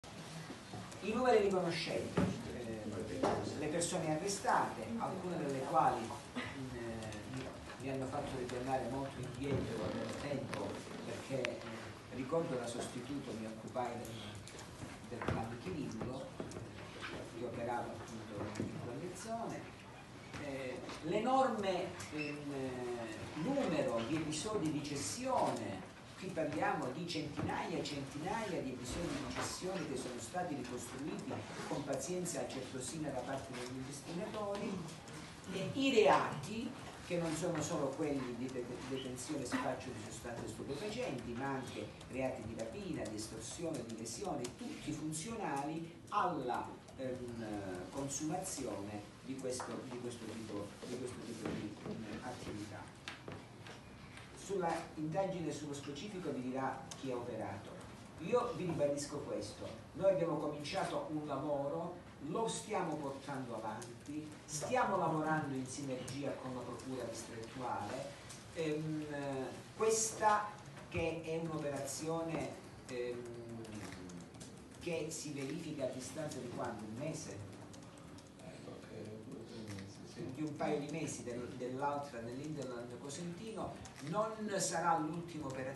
Operazione antidroga nel Savuto, le dichiarazioni del procuratore Spagnuolo
Le dichiarazioni del procuratore capo Mario Spagnuolo durante la conferenza stampa degli arresti eseguiti oggi dai carabinieri nella zona del Savuto.